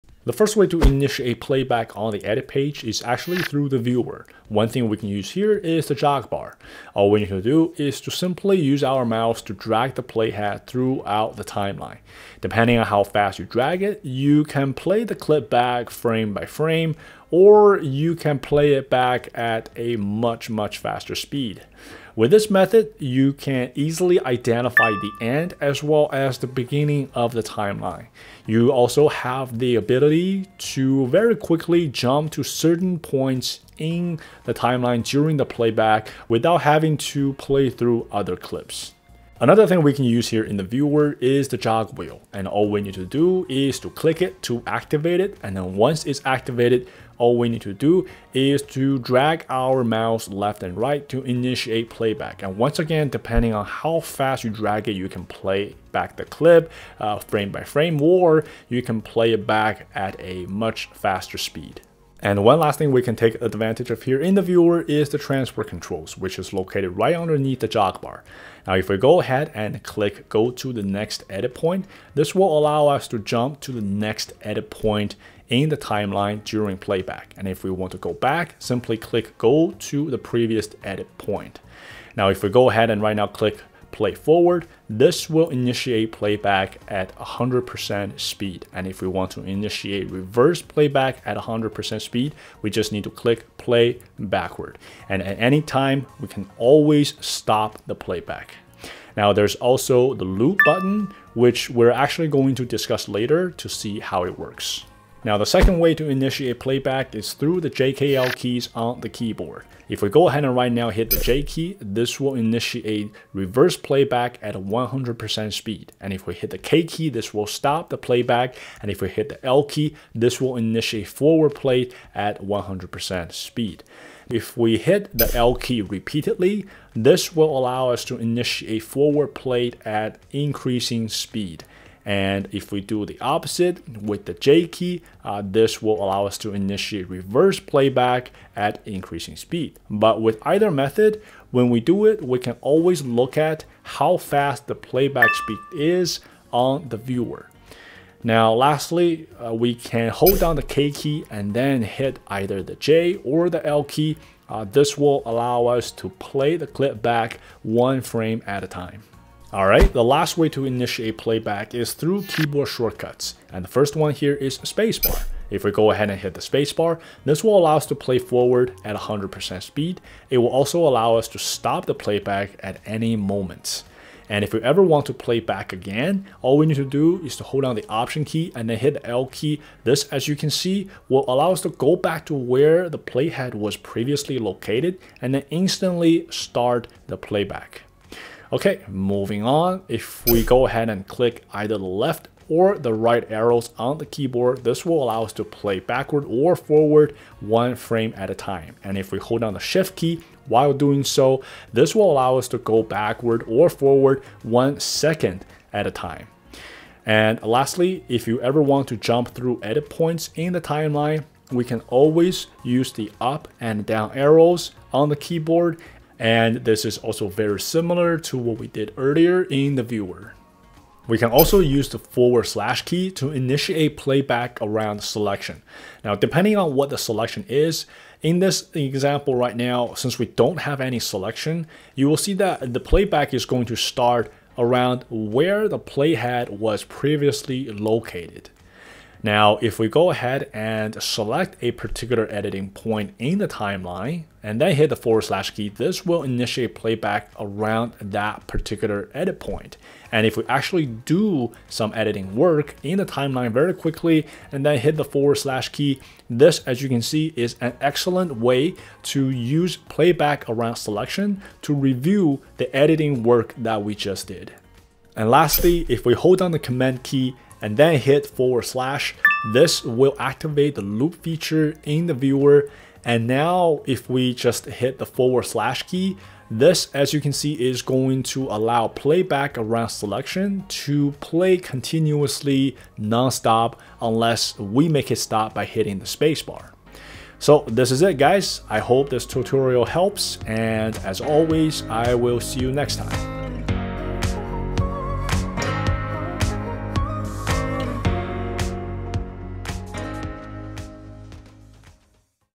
3 Ways to Initiate Playback in DaVinci Resolve (Hidden Features!) and Why You Should Know | Tutorial
Let's really understand this deceivingly easy subject, playback may seem very simple at first glance but if we really dig into it, there are many different ways to initiate it and in DaVinci Resolve, it is no exception. We will look at 3 different ways to start playback in DaVinci Resolve, some interesting hidden features, and how you can apply this depending on your preference or particular scenario.

Key Takeaways:
00:00 Playback Using Viewer (Jog bar, Jog wheel, Transport Controls)
01:47 Playback Using JKL Keys
02:42 Playback Using Keyboard Shortcuts